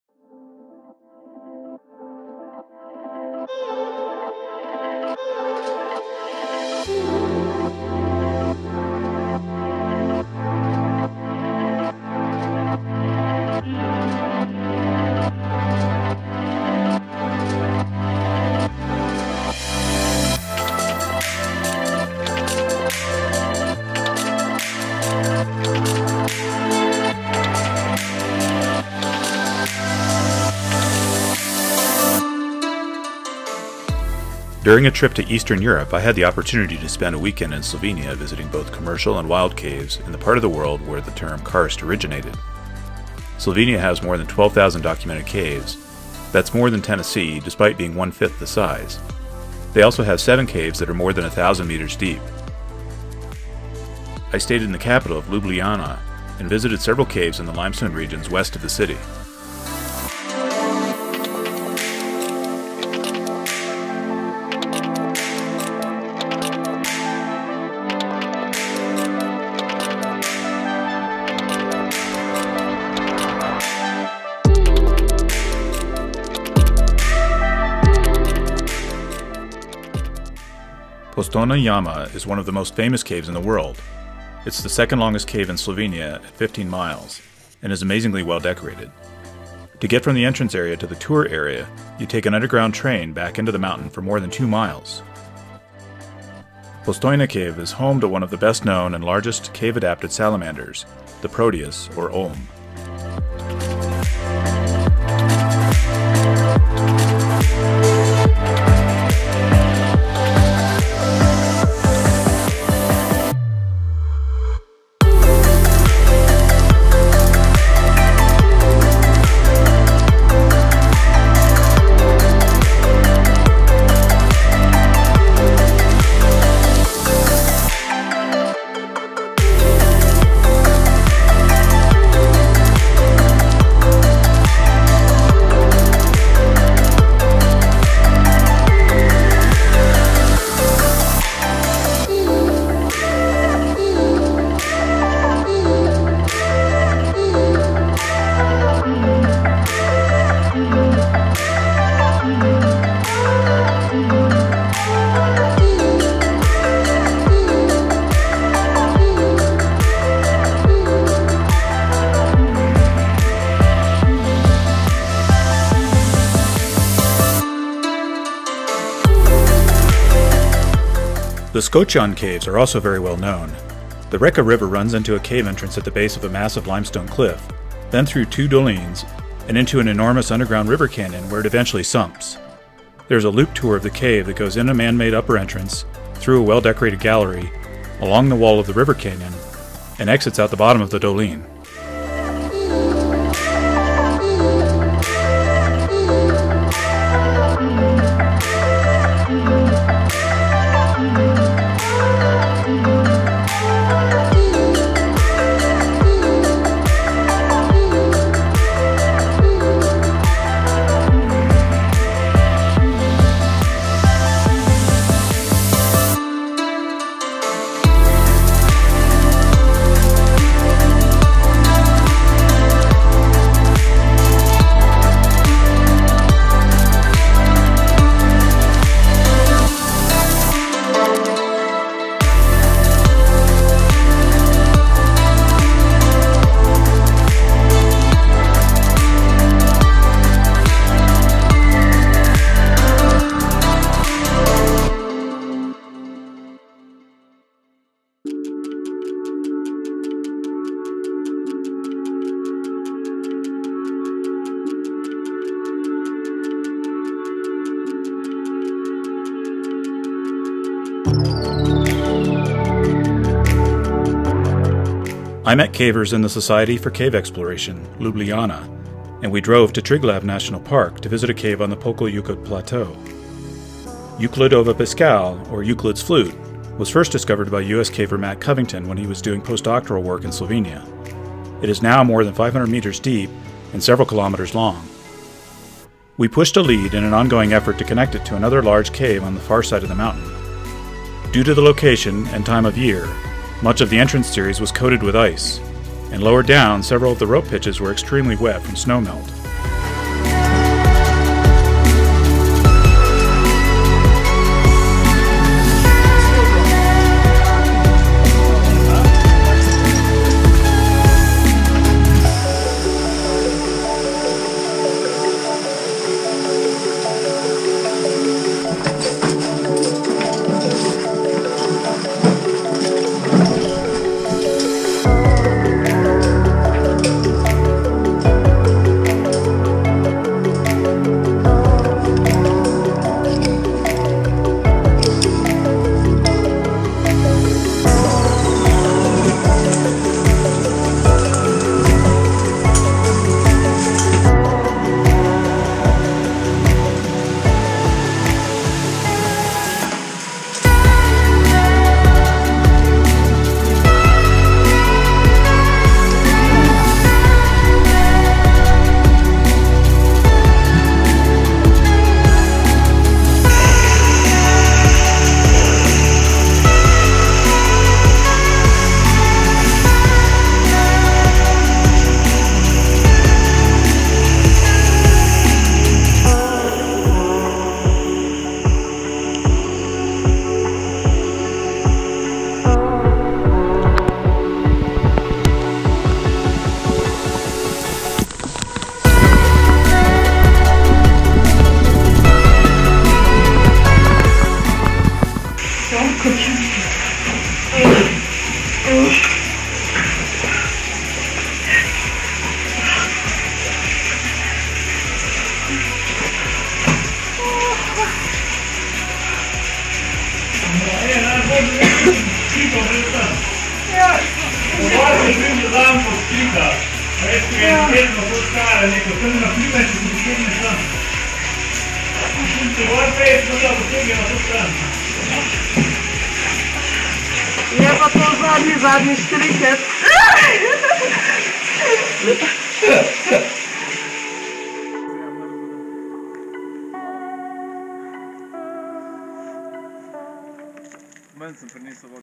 Caving in Slovenia
Commercial tours of Postojna and Skocjan Caves in the Karst region of Slovenia. Also wild caving in Triglav National Park.
For information on wild cave exploration in Slovenia, contact the Society for Cave Exploration Ljubljana